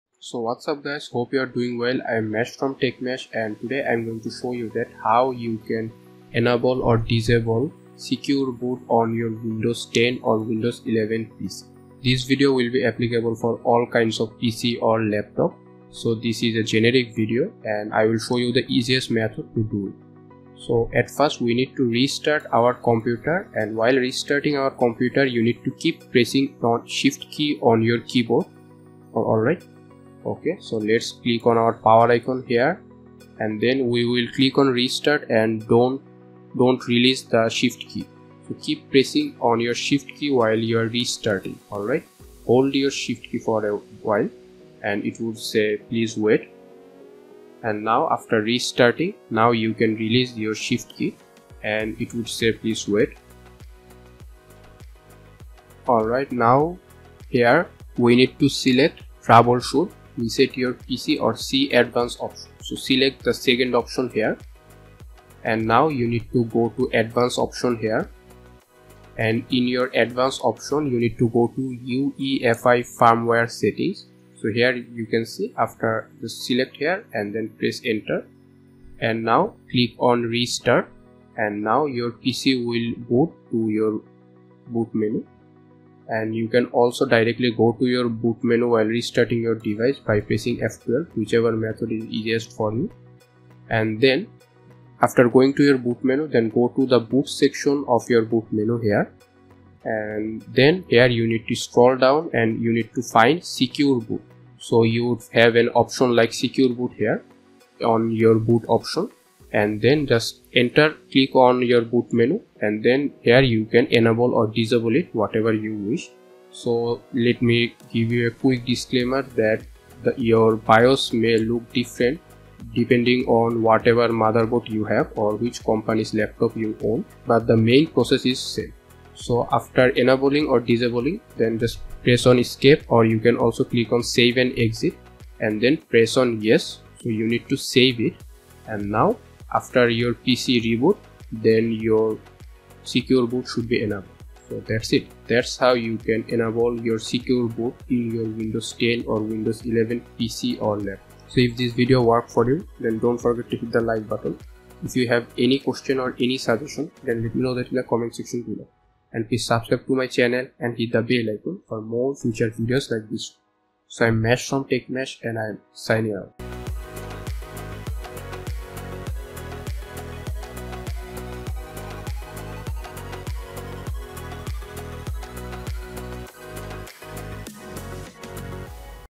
How to Enable/Disable Secure Boot in Any Windows || Easy method
This video will show you how to enable or disable secure boot in Windows 11 using an easy method. Secure boot is a security feature that helps keep your computer secure by preventing unauthorized changes to the operating system.

If you're wanting to learn how to enable or disable secure boot on your Windows 11 computer, this video is for you! By the end of this video, you'll know how to easily disable secure boot and make changes to your computer without any problems!